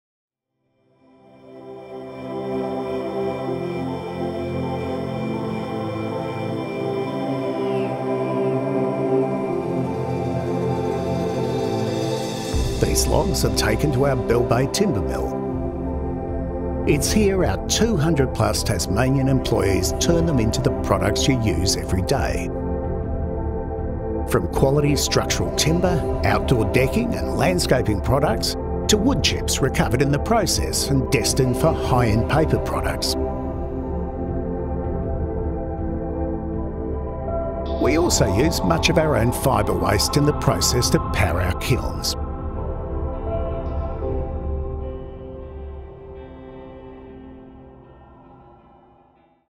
Timberlink - Bell Bay Mill
Our Bell Bay mill employees over 200 Tasmanian's, is high tech and low waste. Find out more here